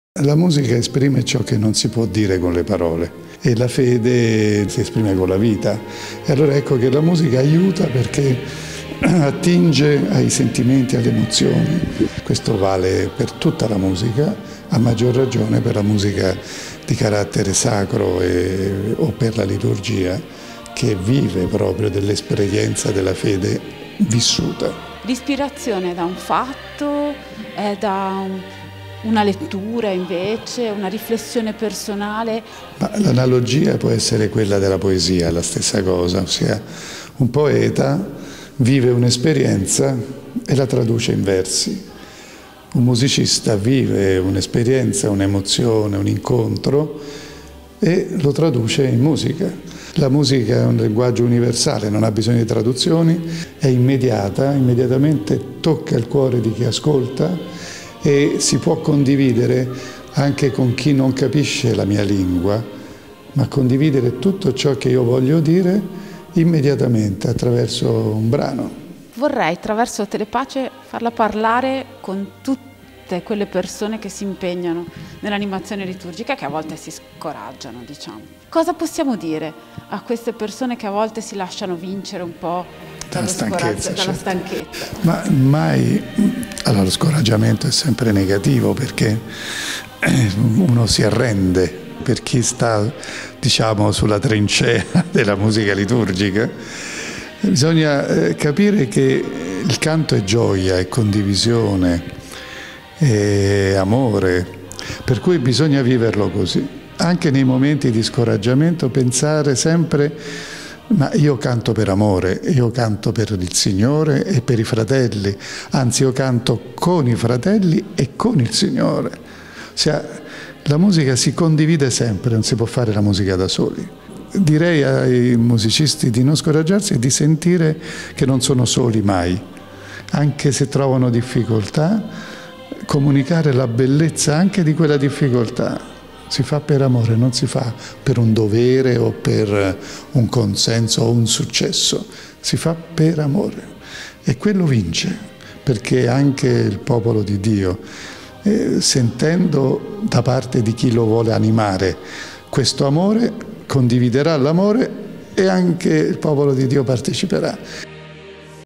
Mons. Frisina: "Si canta per amore, la musica non ha barriere"
Mons. Marco Frisina, compositore dei più noti canti liturgici contemporaneai, Direttore del Coro della Diocesi di Roma, è stato oispite in Diocesi per un incontro formativo che ha riscosso l'interesse di tante persone, impegnate nell'animazione liturgica nelle nostre comunitàNoi lo abbiamo intervistato